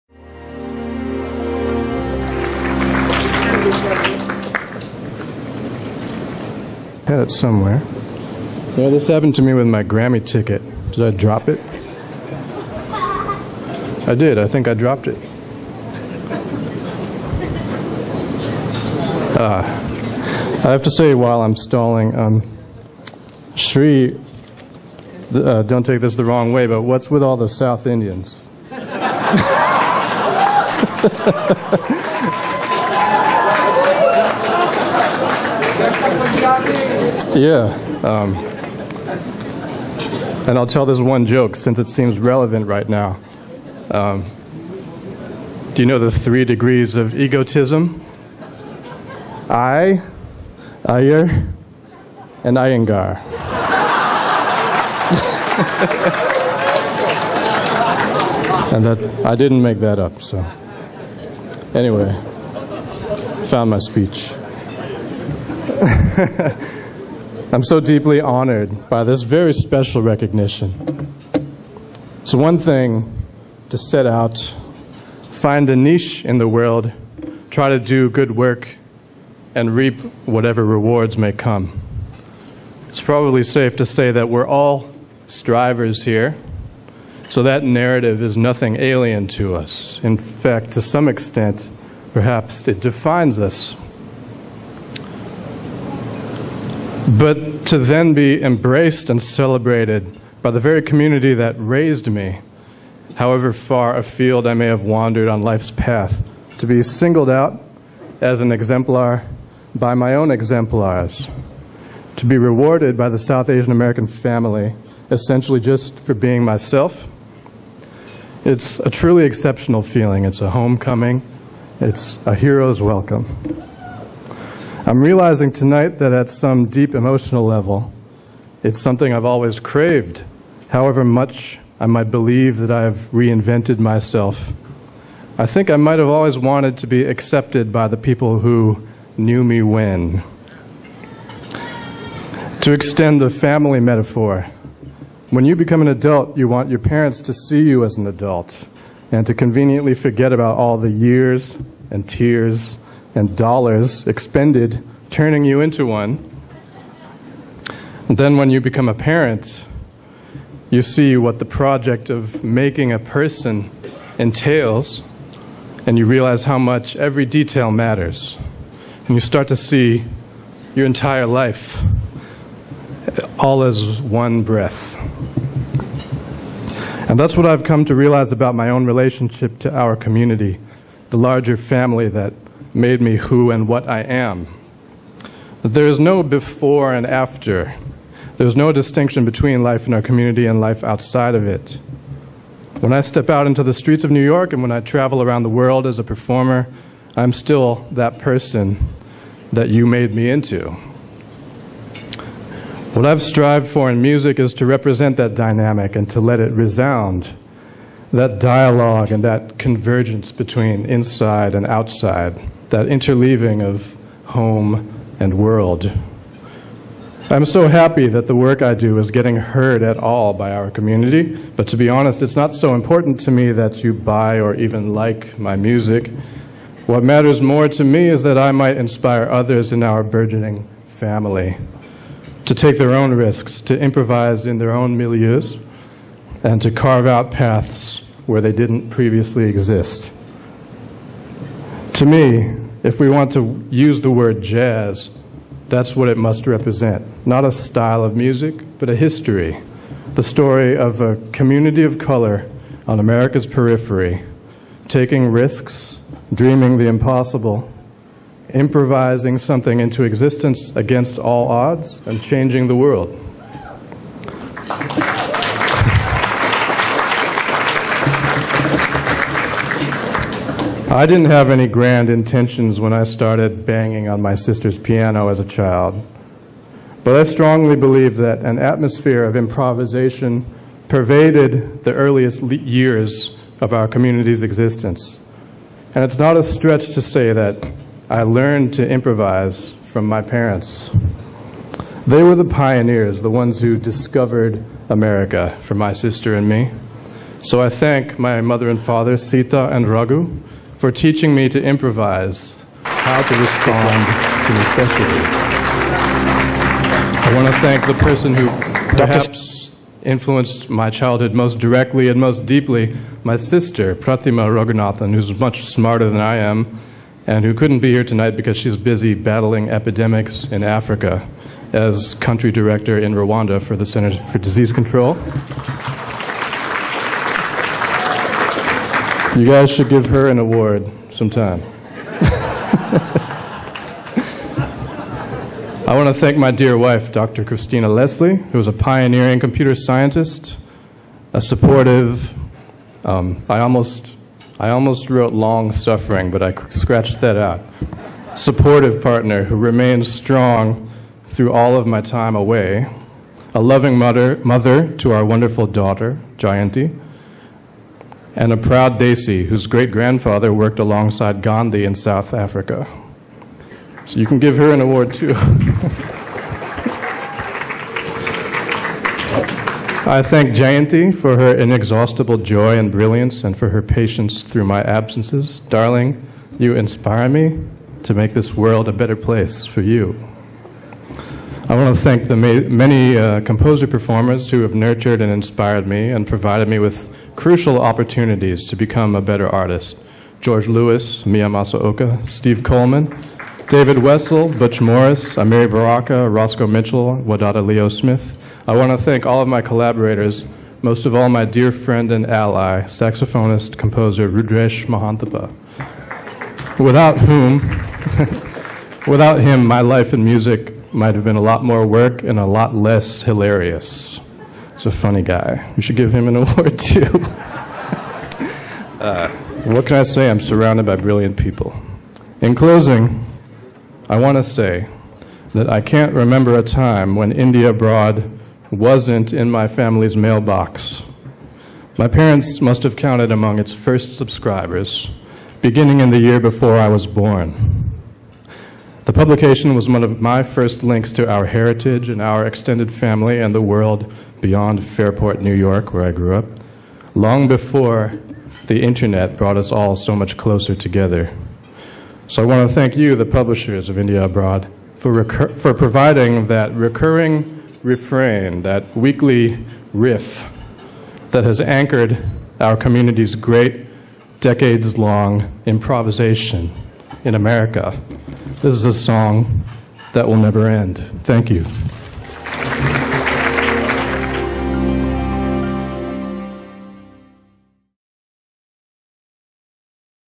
Vijay Iyer Winner of the India Abroad Publisher's Special Award for Excellence
Vijay Iyer wins the India Abroad Publisher's Special Award for Excellence